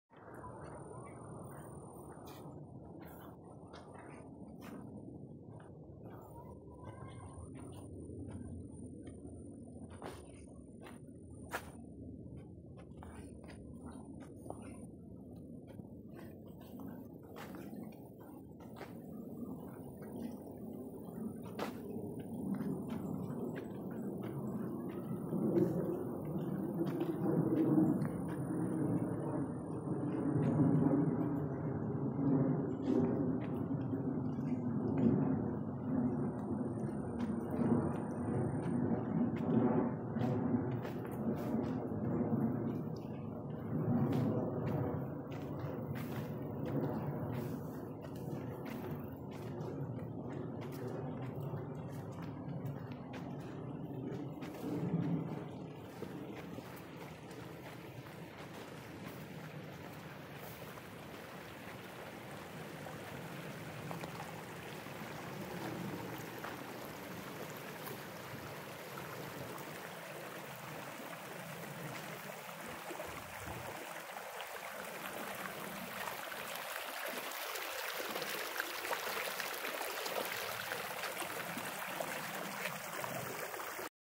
Río caliente en realidad virtual | Episodio #11
Río caliente en el bosque de la primavera en realidad virtual.
El río caliente es un río de aguas termales que se encuentra en el bosque de la primavera en el estado de jalisco en México.